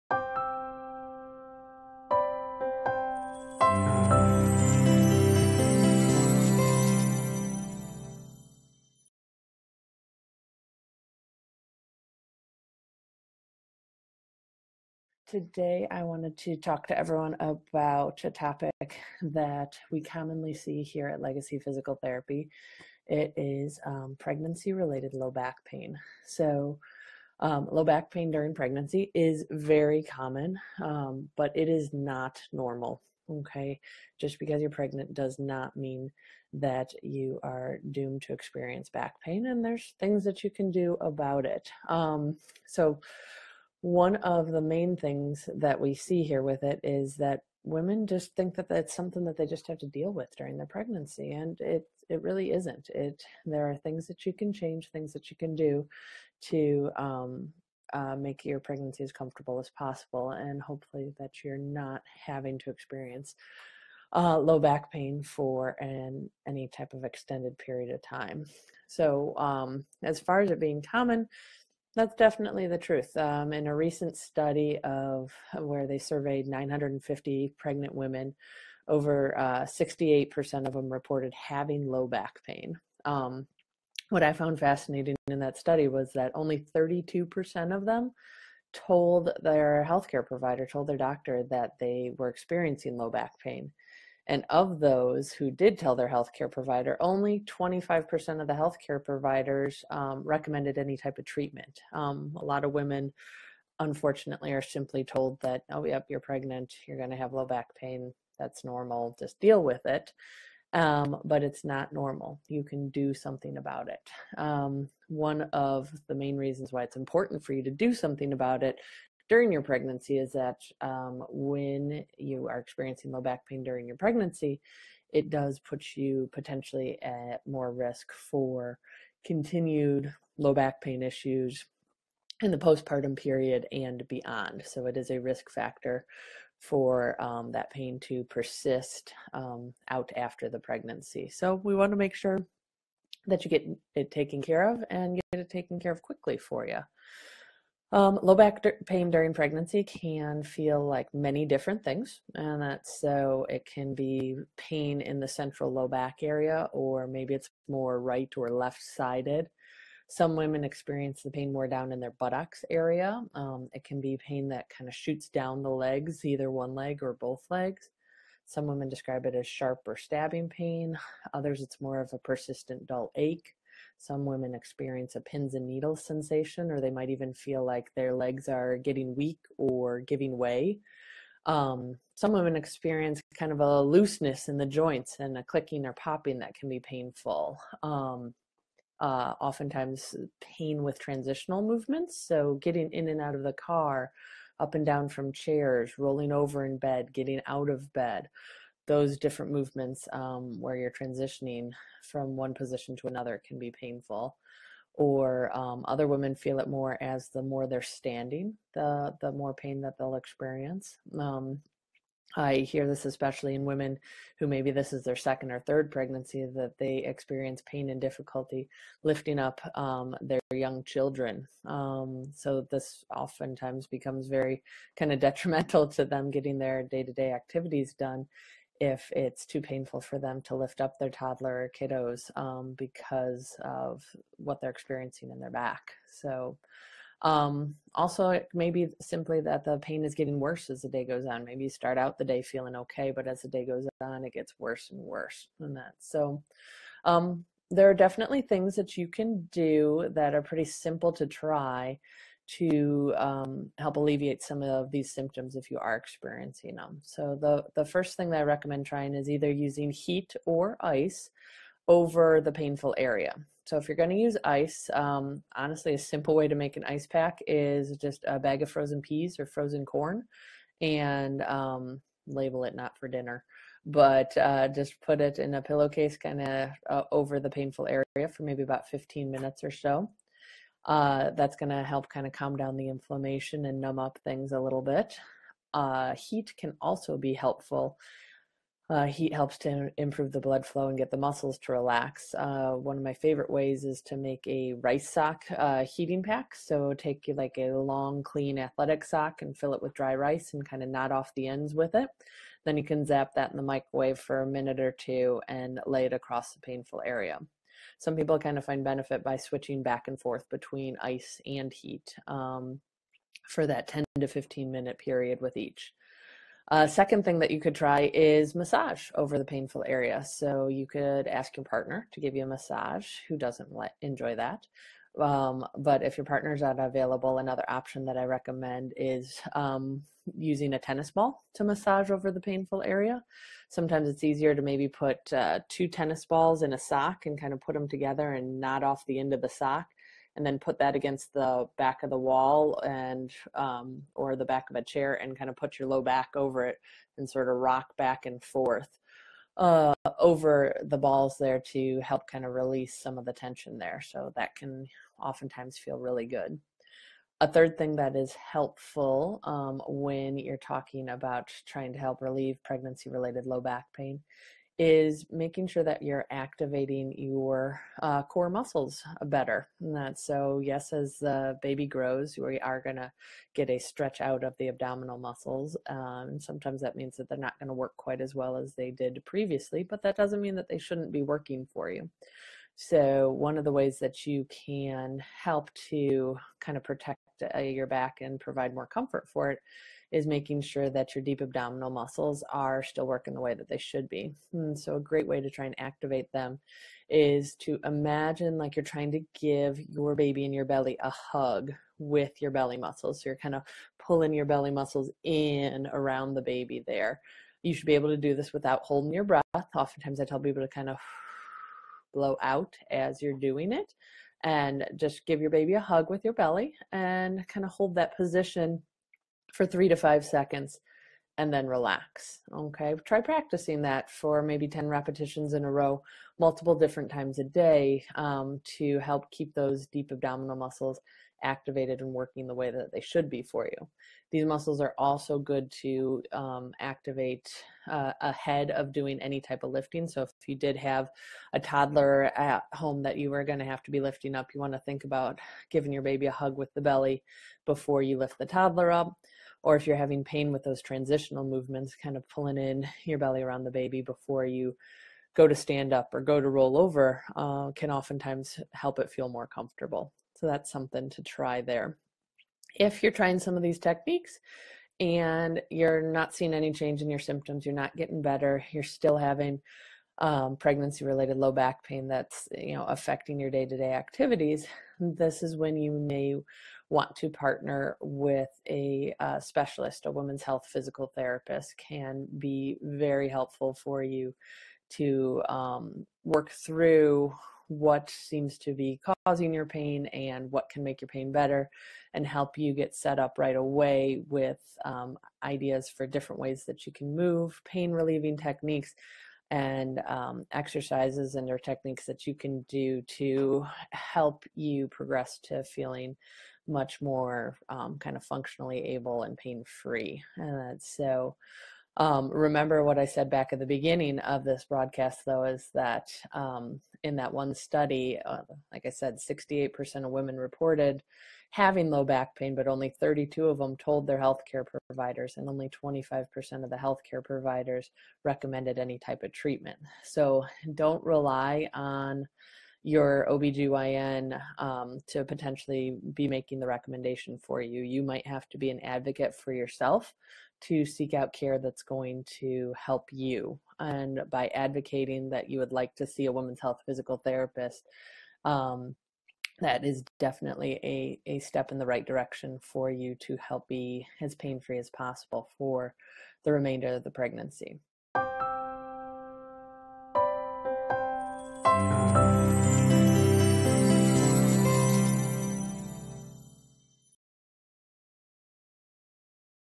Low Back Pain in Pregnancy
Low back pain during pregnancy is very common but it is not normal.  Just because your pregnant does not mean you are doomed to experience back pain and there are things that you can do about it.